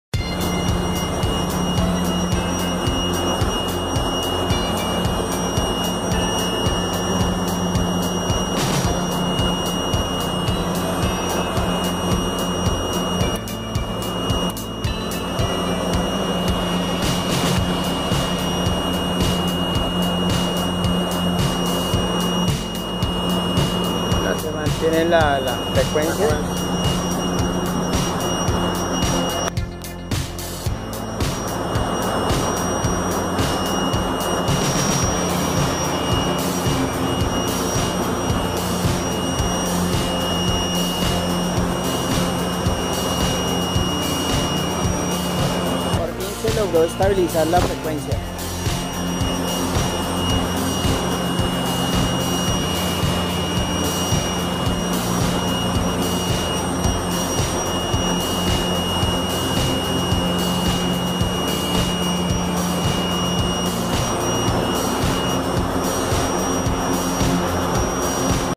Arranque de Horno de Inducción
Arranque horno de inducción de nuestro Cliente Cerromatoso.